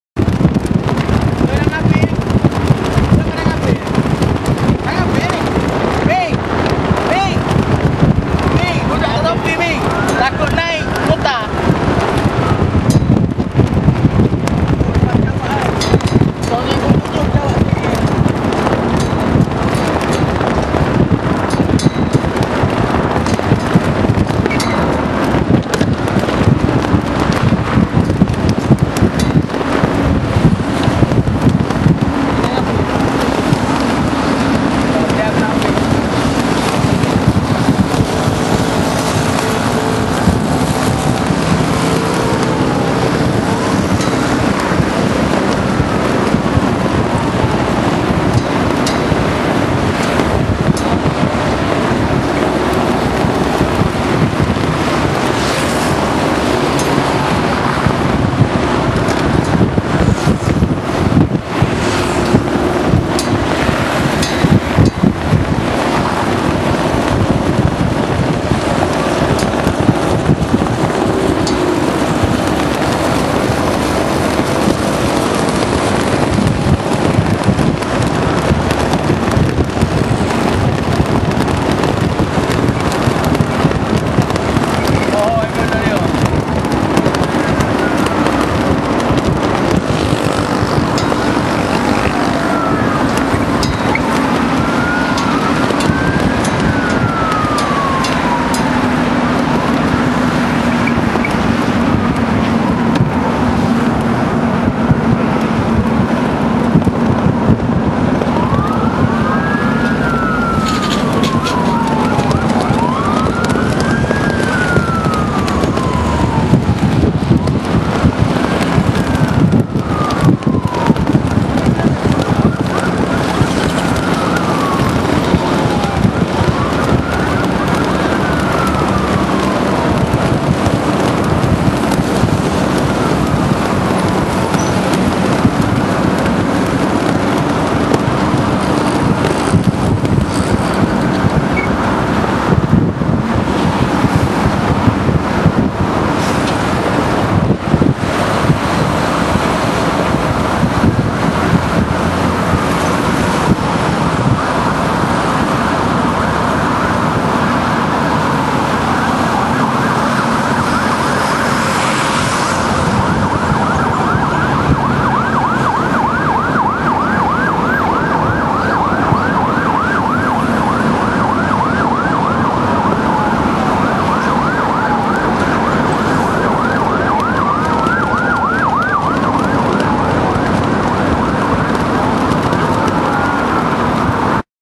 BSM Kelantan - Nur Kasih 2 2011 -30
Kembara Khidmat Sosial Nur Kasih 2 (2011)
20 - 23 Oktober 2011

anjuran bersama
Bulan Sabit Merah Cawangan Kelantan
Rakan Radio Amatur Kelantan